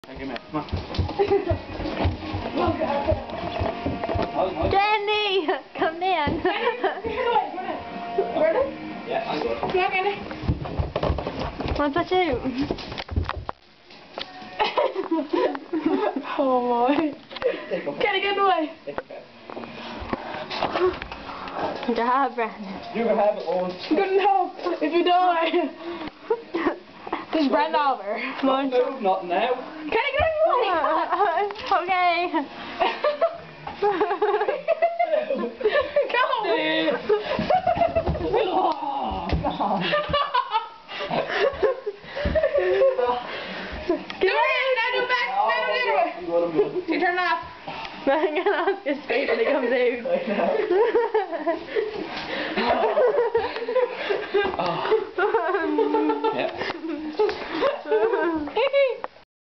Stairs in a Sleeping Bag
Yeah, Brandon Oliver was here one day, and wanted a sleeping bag, so he could go down the stairs.. it was funnier if you were there, but yeah, its still funny on this.